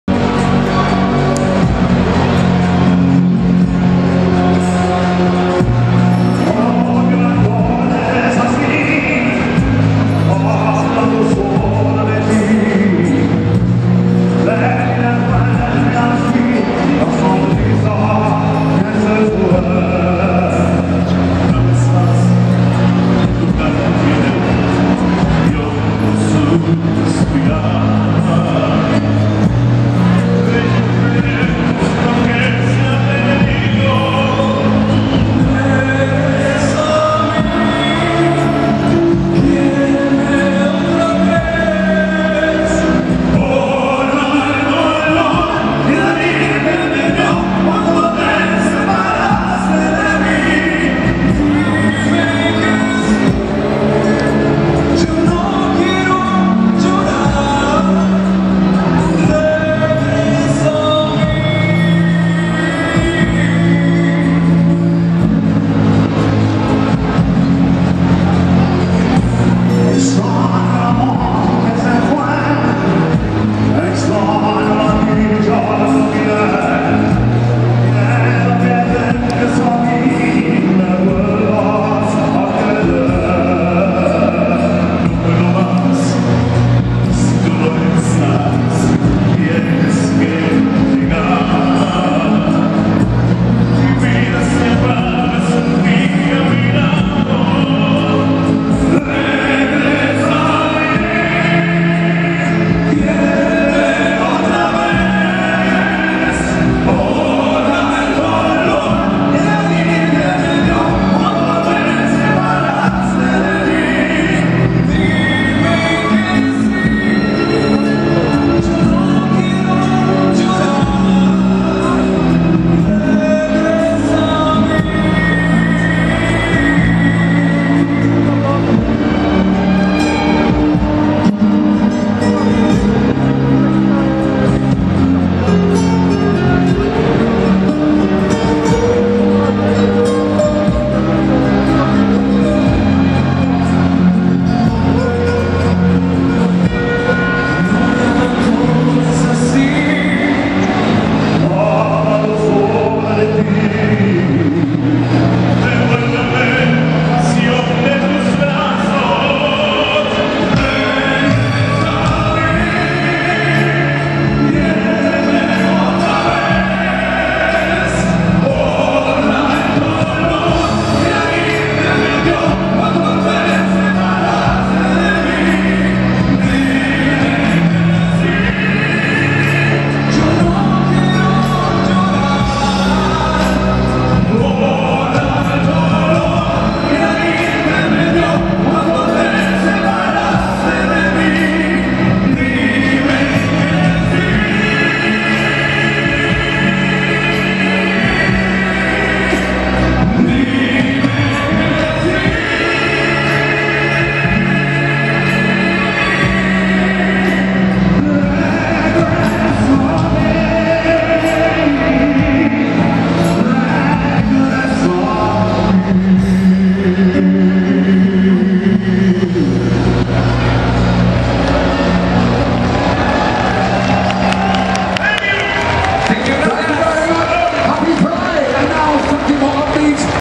The Three Tenors - Gay Pride Maspalomas 2014
The Three Tenors en la gala para ACES (Amigos contra el Sida) del Gay Pride Maspalomas de Gran Canaria. 22/5/2014